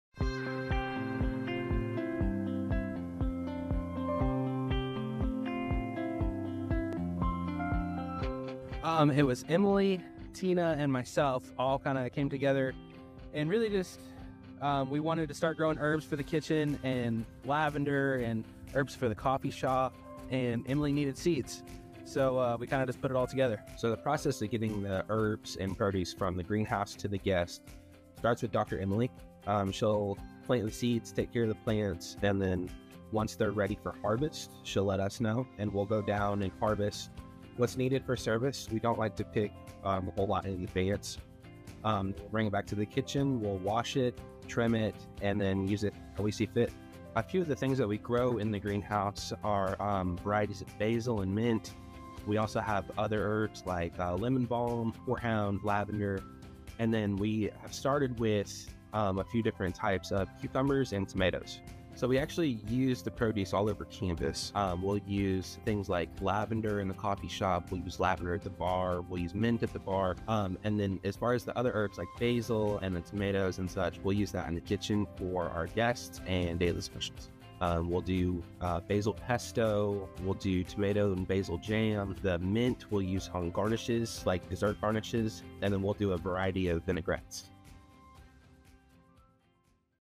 Growing Our Garden
Explore how the Institute uses its greenhouse to support new culinary projects and creations!